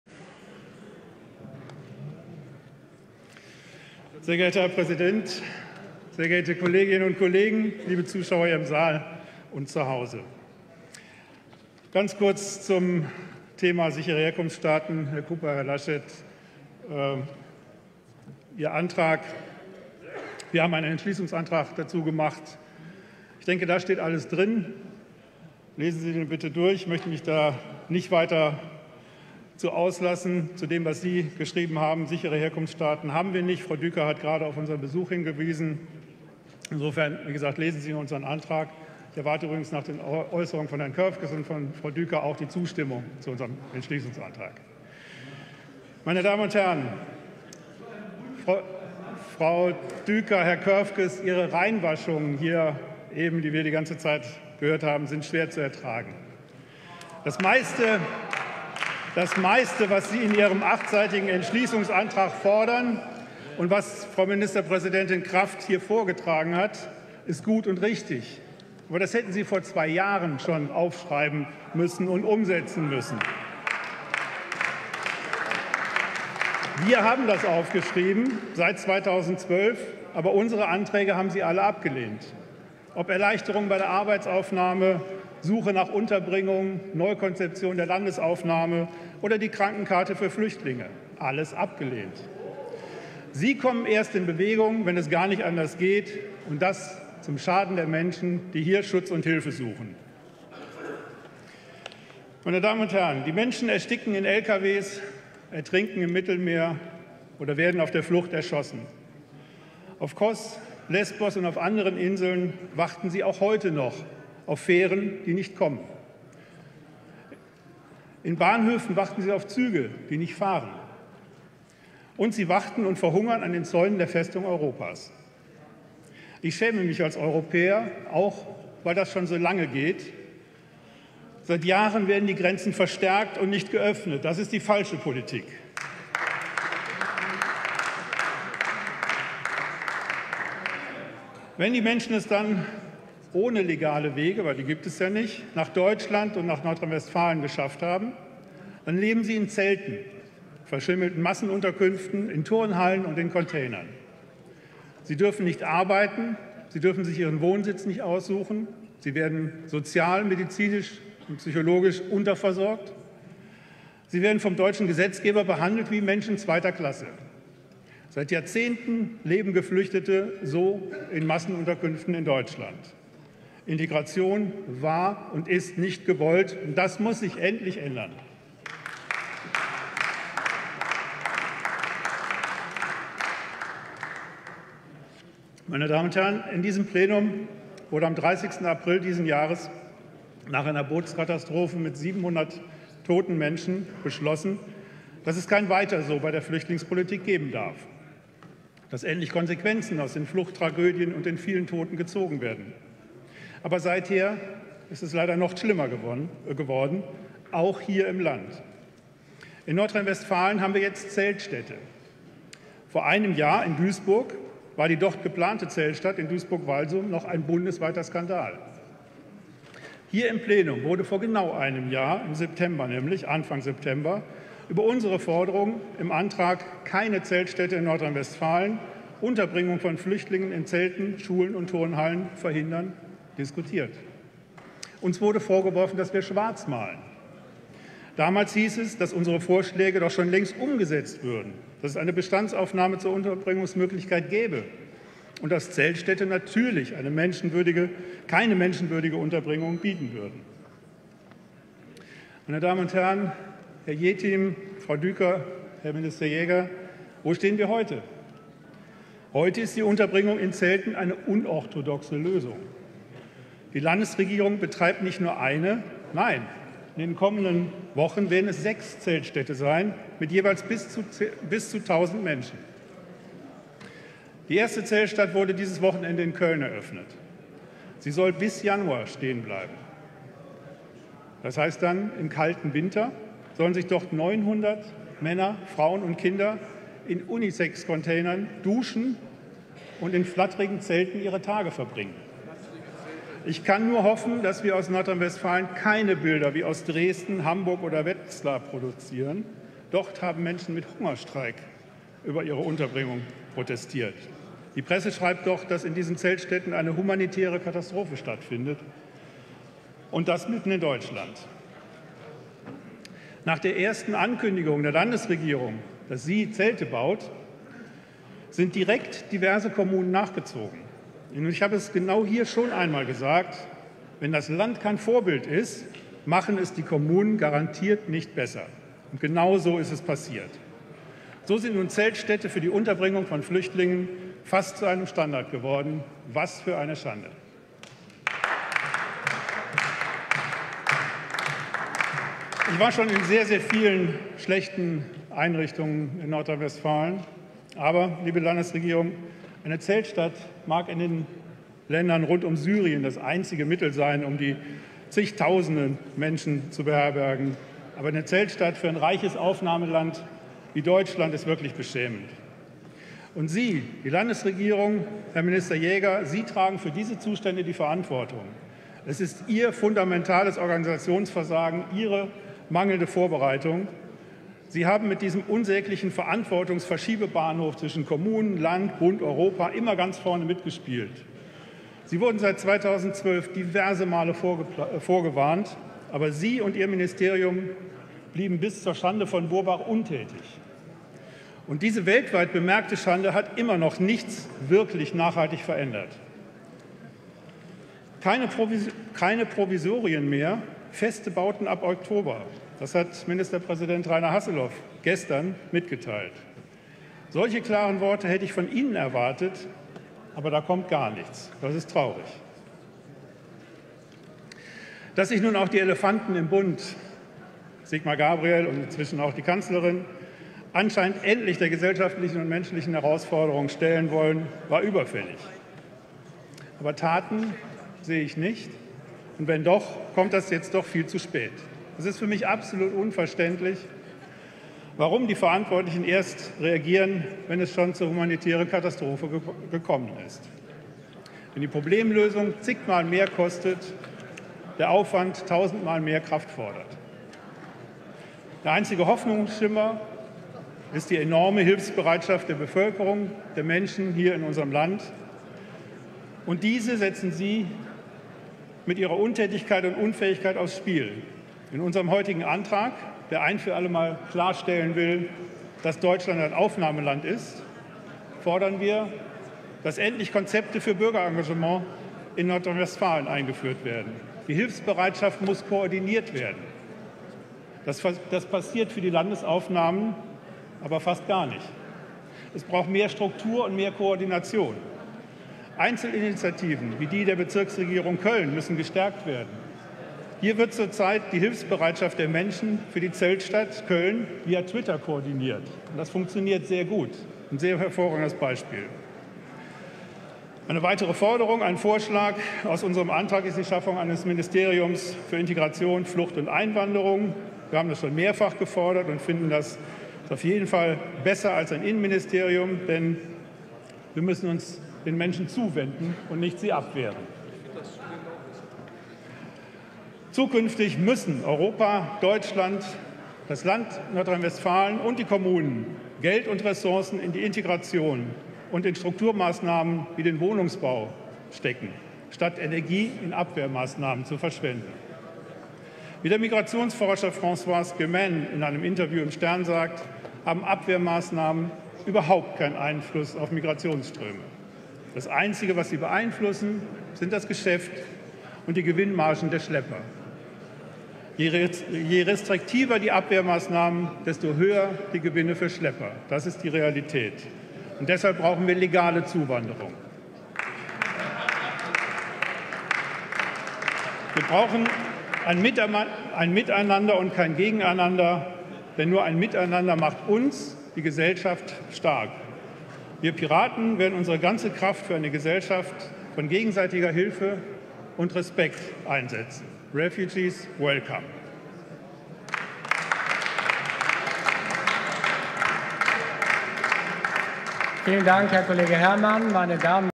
Frank Herrmann zu Flüchtlingsproblematik in NRW
Unser Antrag im Plenum (02.-04. September 2015)

Aus der Vergangenheit lernen: Nordrhein-Westfalen muss sich der politischen Verantwortung als Aufnahmeland stellen!